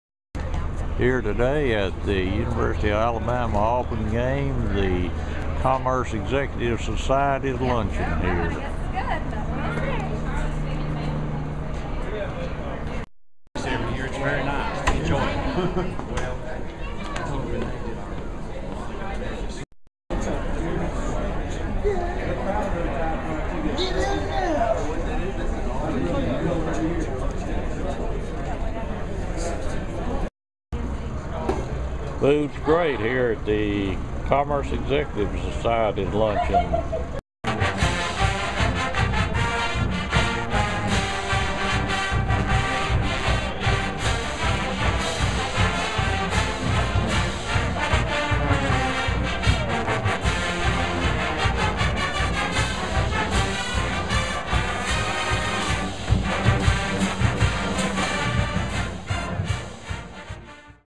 U of A Commerce Executive Society ~ Pre Auburn Game 2012
Met old friends and enjoyed a great meal of B-B-Q Pork & Smoked Wings before Iron Bowl Game!  Enjoyed every meal before each home game this year ~ great benefit of belonging to the Commerce Executive Society. -- beautiful day --0 Alabama won 40 - 0